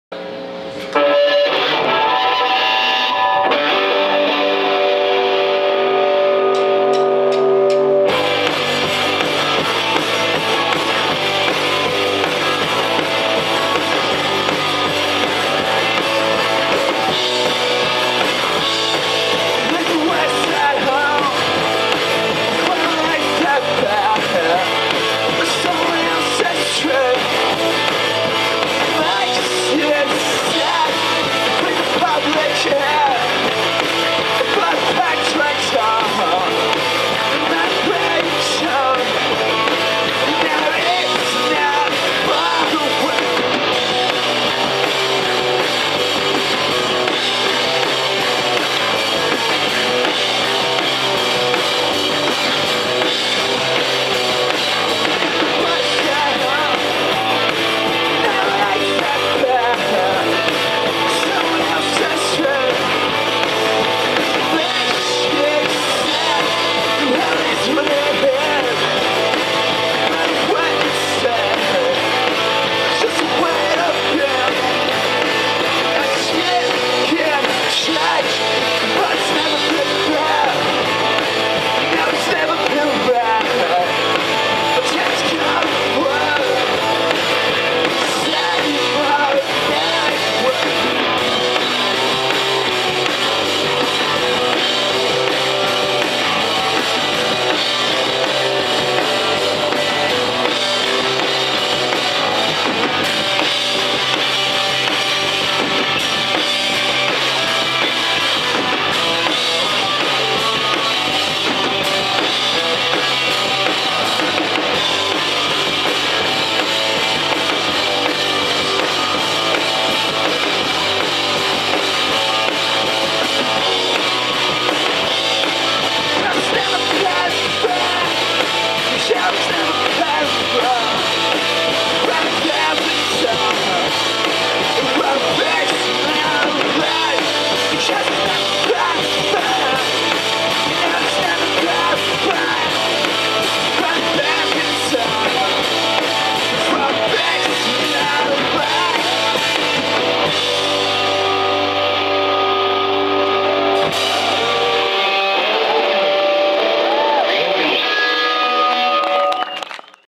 Slow Learners (June 29/2013) Grand Forks, BC
Slow Learners at the Canada Day Weekend Concert in Grand Forks, BC June 29/2013. Put on by Waterlow Audio. Filmed by Megan Kienas.